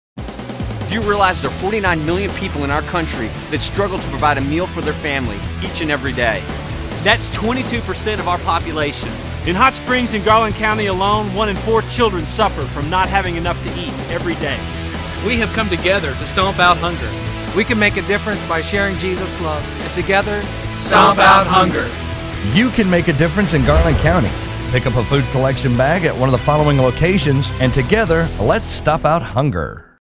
Stomp Out Hunger Commercial HD.mov
A 30 second commercial which will air on television, promoting Stomp Out Hunger in Hot Springs.